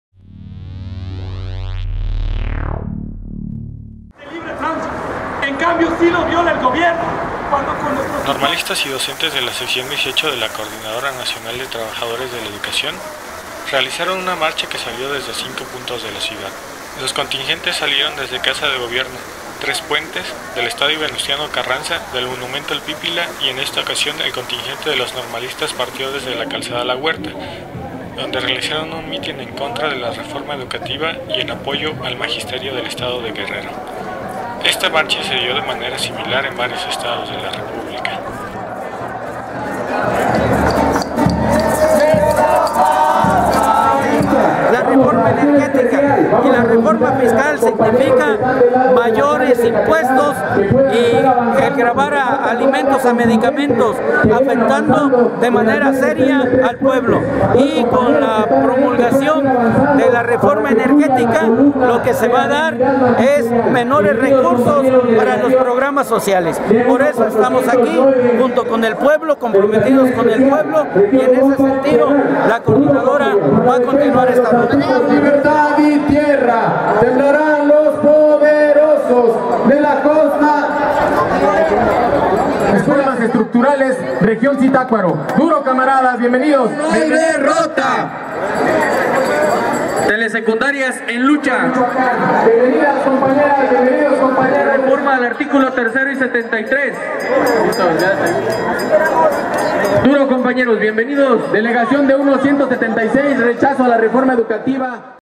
Mega marcha de la sección XVIII en contra de reformas constitucionales
Normalistas y docentes de la Sección XVIII de la Coordinadora Nacional de Trabajadores de la Educación (CNTE) realizaron una marcha que salió de cinco puntos de la ciudad.

Los contingentes saldrán de Casa de Gobierno, Tres Puentes, del Estadio Venustiano Carranza, del Monumento al Pípila y en esta ocasión el contingente de los normalistas partió de la Calzada la Huerta. Para luego coincidir en Palacio de Gobierno, donde realizaron un mitin contra la reforma educativa y en apoyo al magisterio del estado Guerrero.

Esta marcha se dio de manera similar en varios estados de la República.